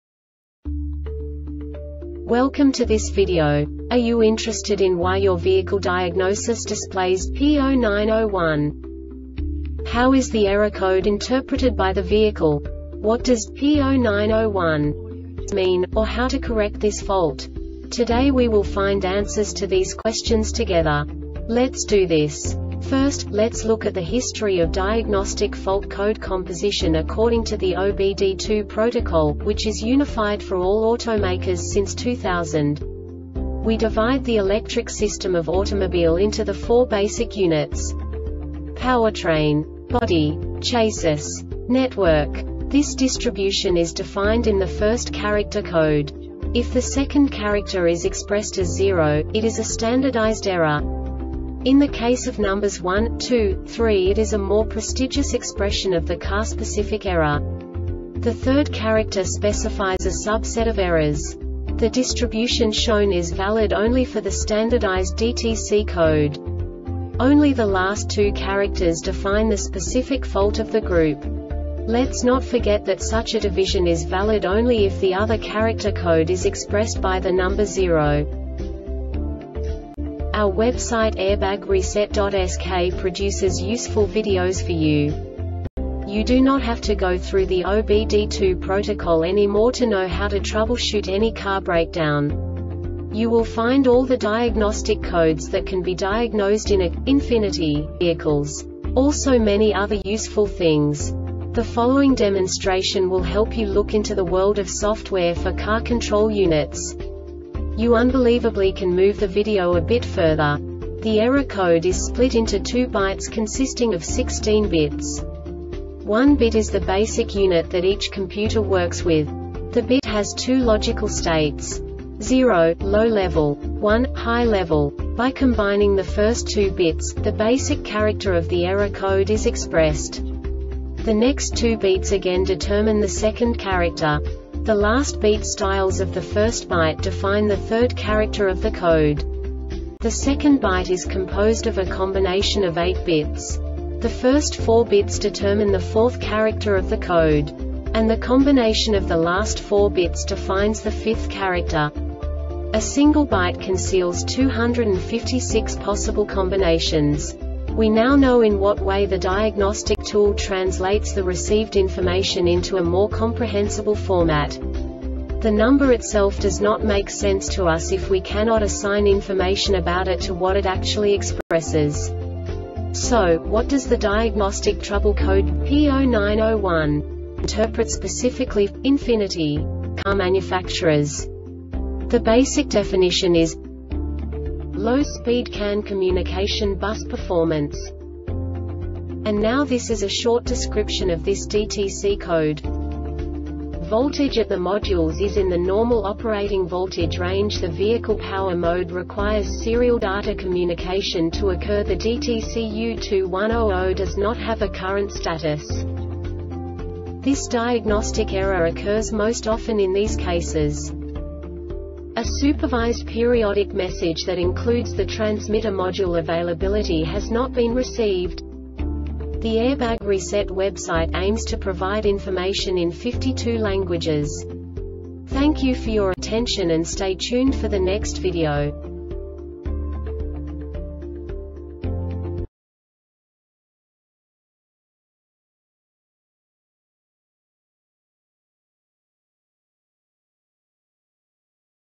DTC Ford P0901 Short Explanation
Contents:
0:21 Basic DTC analysis according to OBD2 protocol standard.
1:48 Insight into programming
2:58 A diagnostically understandable description of the Diagnostic Trouble Code
3:05 Basic error definition

Make:
Ford
Code:
P0901
Definition:
Low Speed CAN Communication Bus Performance
Description:
Voltage at the modules is in the normal operating voltage range.The vehicle power mode requires serial data communication to occur.The DTC U2100 does not have a current status.